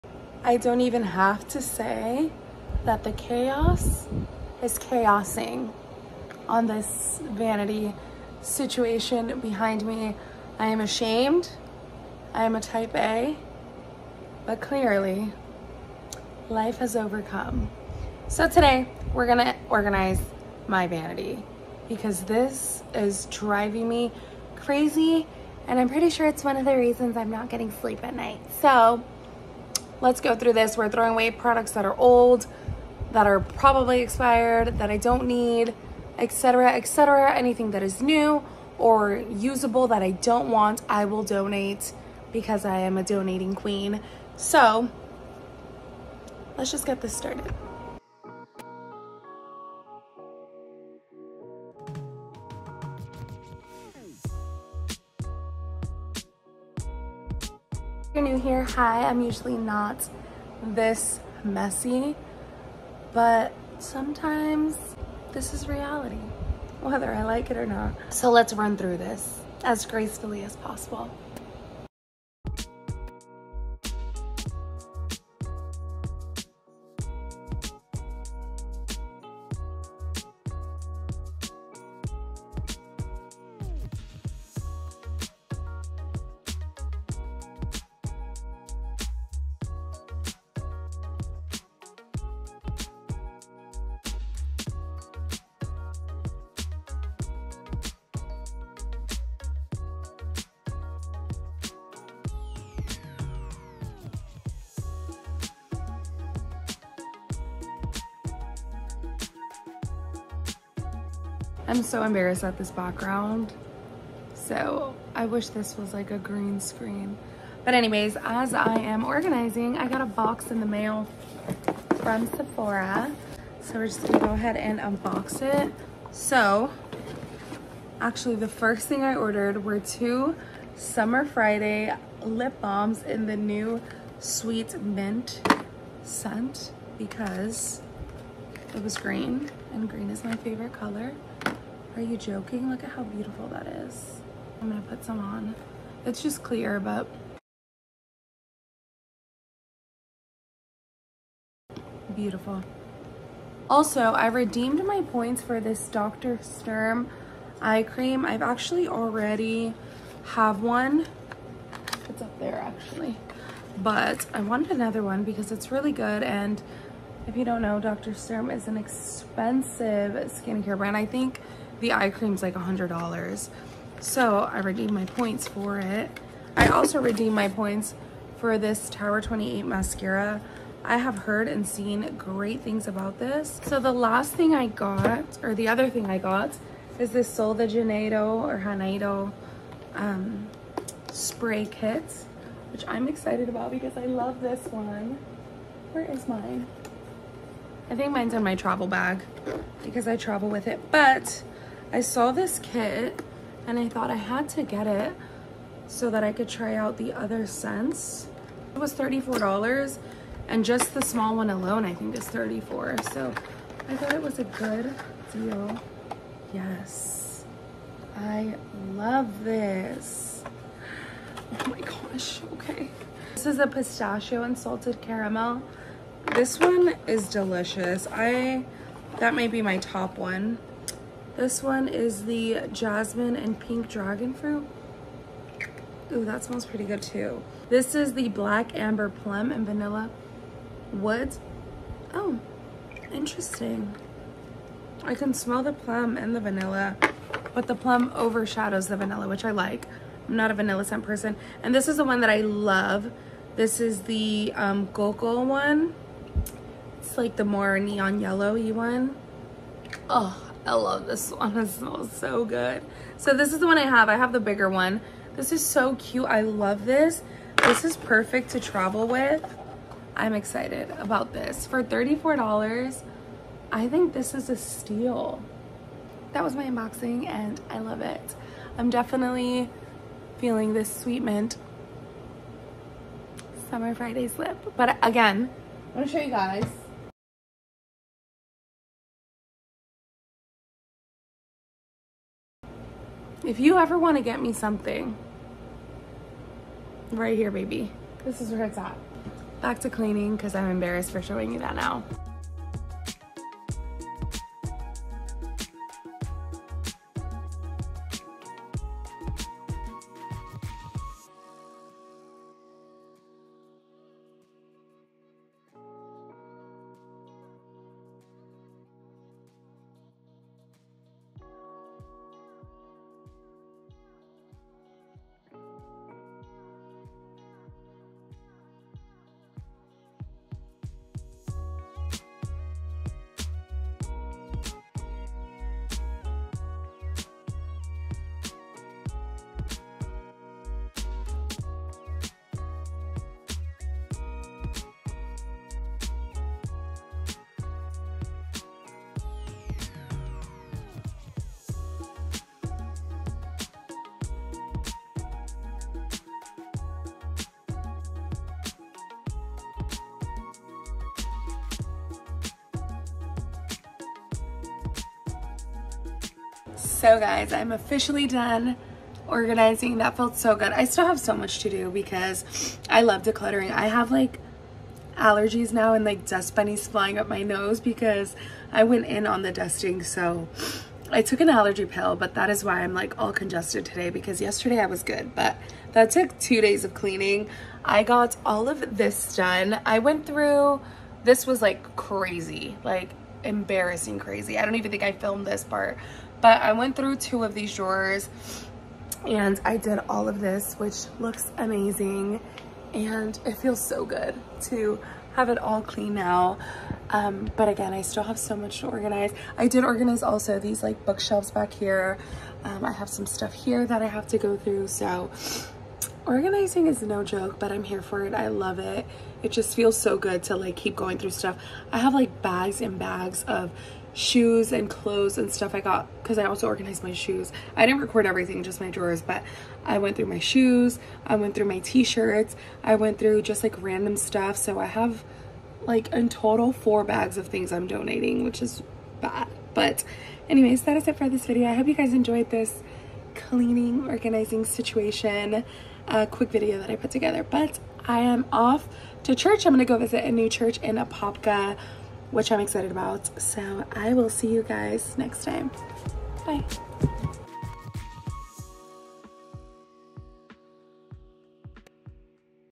Decluttering my Vanity | Sephora Unboxing | justineivette
Hi friends ✨🩷 Welcome! If you’re anything like me, you enjoy watching other people clean so in today’s vlog, I go through my vanity and dressers to declutter and organize a much needed chaotic mess 😅

Tiktok & IG:  @justineivette

“in His presence, there is fullness of joy” - Psalm 16:11